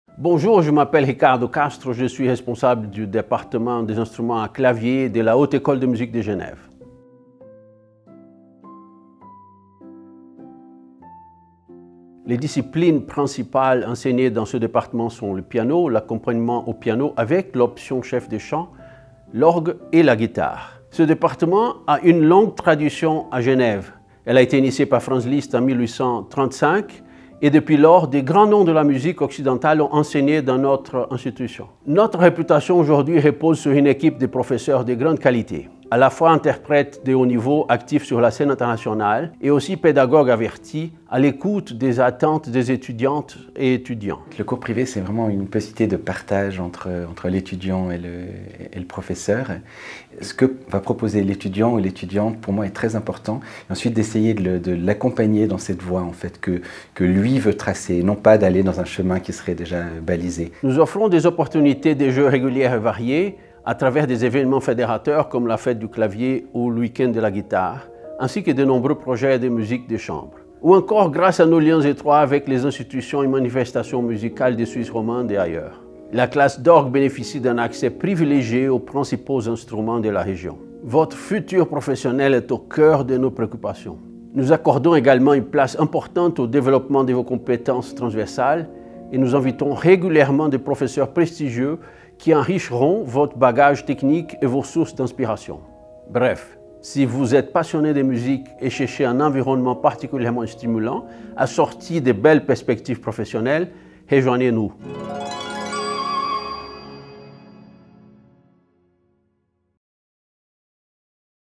Le département des instruments à clavier de la HEM
Ricardo Castro présente le département des instruments à clavier de la Haute école de musique de Genève (HEM)
Réalisation : Upload Productions / Guillaume Carel

Le département des instruments à clavier de la Haute école de musique de Genève (HEM) perpétue une grande tradition d'enseignement du piano et de l'orgue à Genève, initiée par Franz Liszt en 1835.

Les classes de piano ont compté parmi les professeur-e-s des noms aussi illustres que Dinu Lipatti, Nikita Magaloff ou Maria Tipo et demeurent aujourd'hui encore un secteur particulièrement réputé de la HEM.

Aujourd'hui, notre réputation repose sur une équipe de professeur-e-s de grande qualité, à la fois interprètes de haut niveau et pédagogues avertis, attentifs aux attentes des étudiant-e-s. Nous offrons une approche pédagogique très ouverte, permettant à toute et tous de travailler avec plusieurs professeur-e-s de la discipline principale. Cela est presque unique au monde et offre à nos étudiant-e-s une opportunité fantastique de multiplier les perspectives d'apprentissage.

Le département organise régulièrement des événements fédérateurs tels que la Fête du clavier ou la Fête de la guitare, ainsi que de nombreux projets de musique de chambre. Nos liens étroits avec les institutions et manifestations musicales de Suisse romande offrent des opportunités de jeu nombreuses et variées à nos étudiant-e-s. La classe d'orgue bénéficie également d'un accès privilégié aux principaux instruments de la région.

Grâce à une approche pédagogique novatrice, des événements transversaux excitants et des rencontres avec des maîtres du monde entier, le département offre un environnement d'apprentissage stimulant, flexible et diversifié pour les étudiant-e-s passionné-e-s de musique.

Les disciplines principales enseignées au sein du département sont le piano, l'accompagnement au piano, comprenant une option de chef-fe de chant, l'orgue et la guitare.